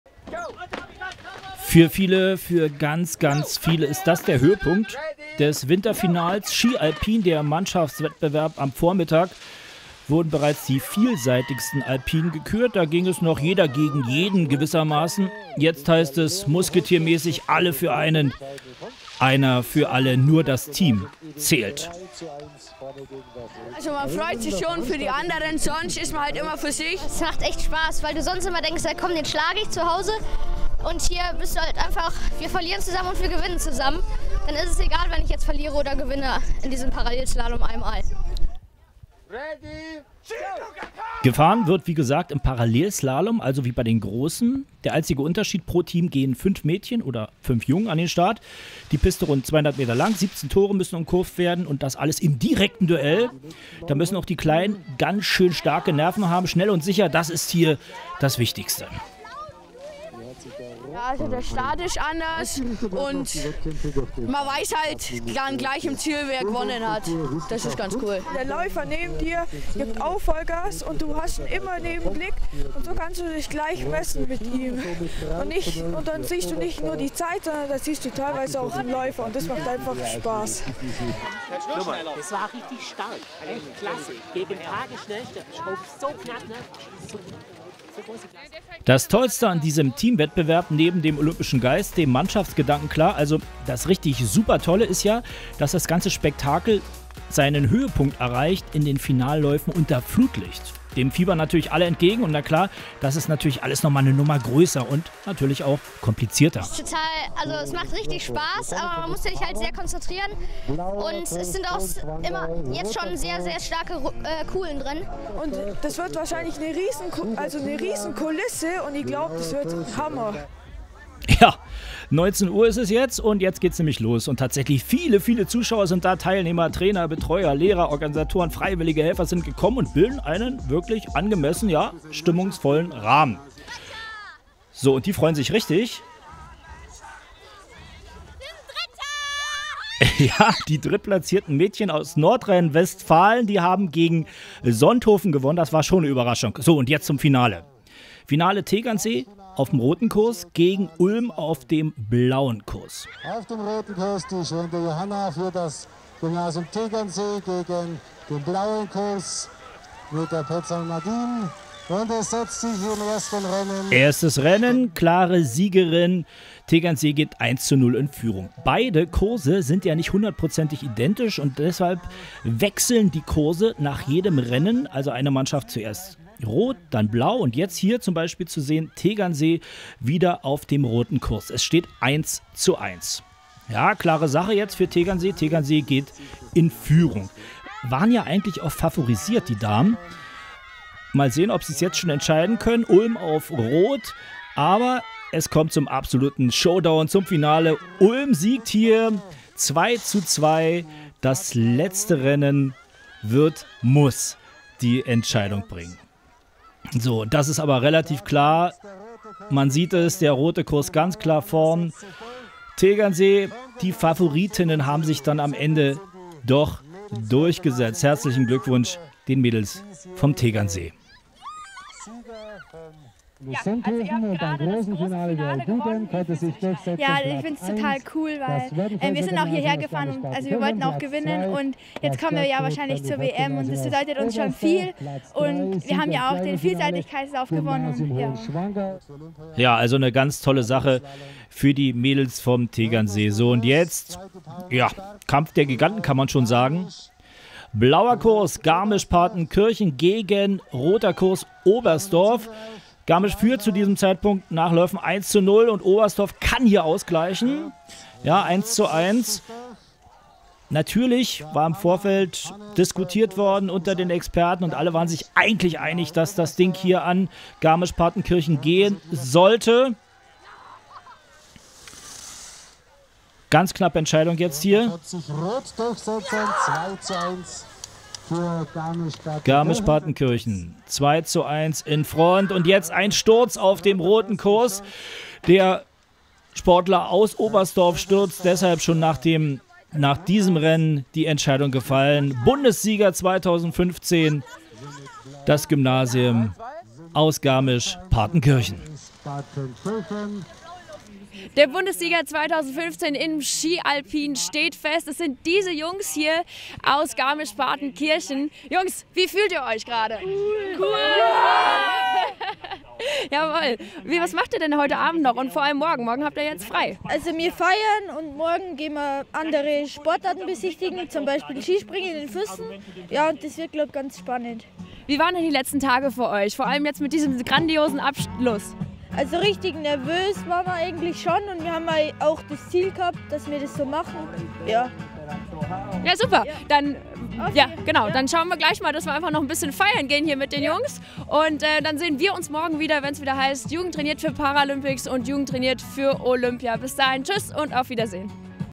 JTFO / JTFP | Winterfinale 2015 – Tag 2 | Parallelslalom
Am Vormittag waren die Kids von JTFO noch allein auf der Piste unterwegs. Am Abend dann das große Highlight. Der Teamparallelslalom unter Flutlicht vor beeindruckender Kulisse. Der Höhepunkt des zweiten Wettkampfstages.